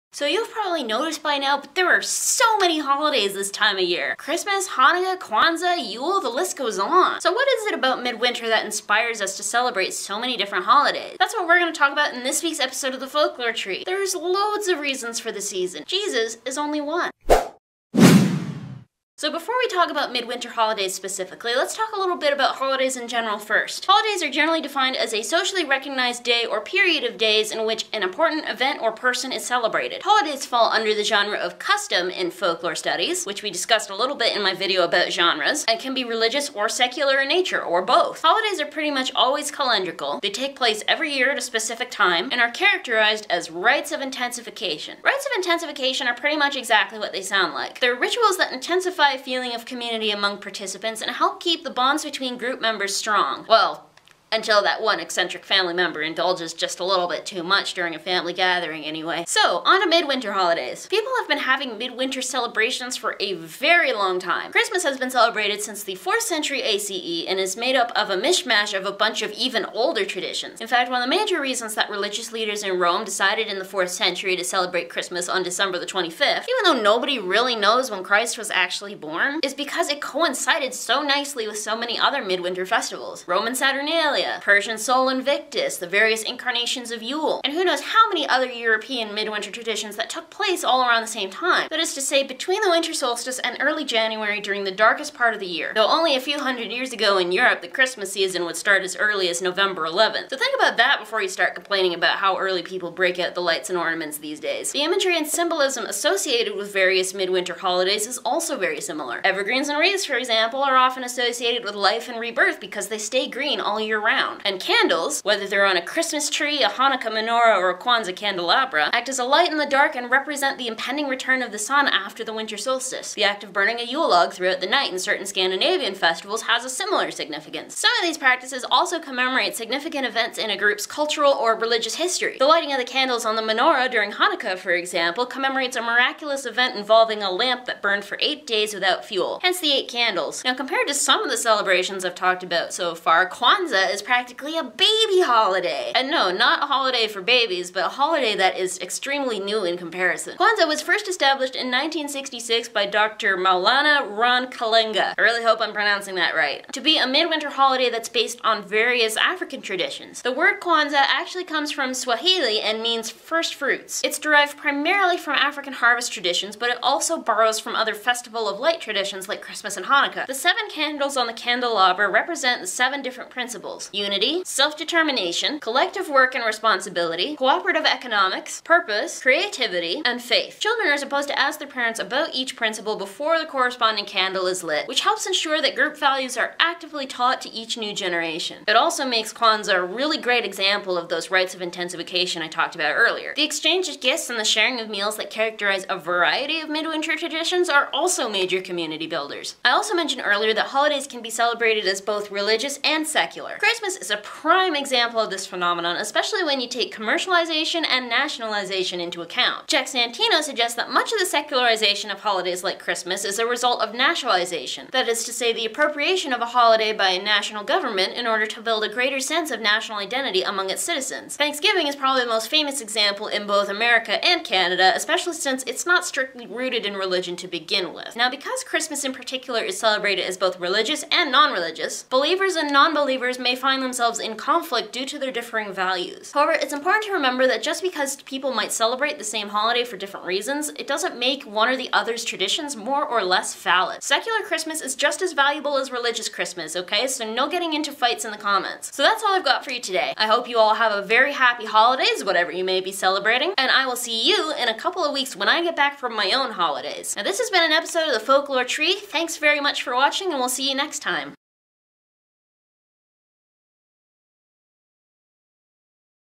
The Folklore Tree #9: Midwinter Holidays
In the spirit of the season, here's an episode on midwinter holidays!

Citations:

Brodie, Ian. "Custom." Class lecture, Cape Breton University, Sydney, Nova Scotia, 2013.

Santino, Jack. "Autumn into Winter: Celebrating Death and Life." In All Around the Year: Holidays and Celebrations in American Life, 142-209. University of Illinois Press, 1994.